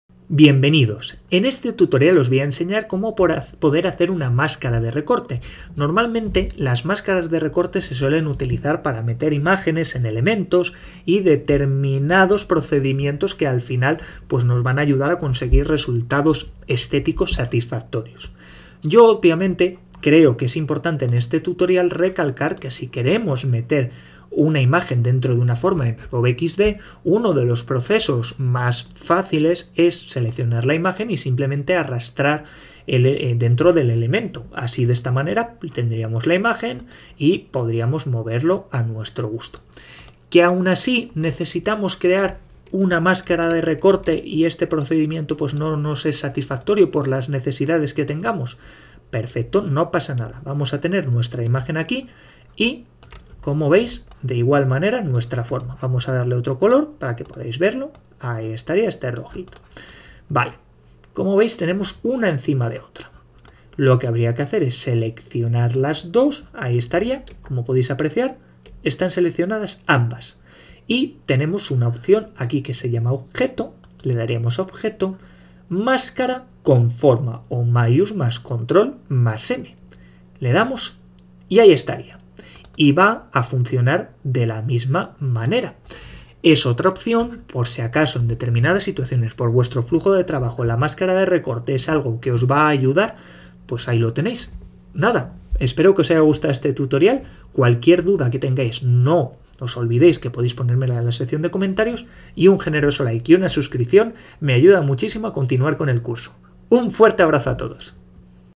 MÁSCARA DE RECORTE en ADOBE XD 🔥 - ADOBE XD TUTORIAL
👉 En este vídeo aprenderemos a crear una máscara de recorte en Adobe XD. Las máscaras, también llamadas máscaras con forma son un elemento de Adobe XD muy útil que nos puede ayudar a crear determinados efectos.

Crear una máscara de recorte en Adobe XD en este tutorial en español.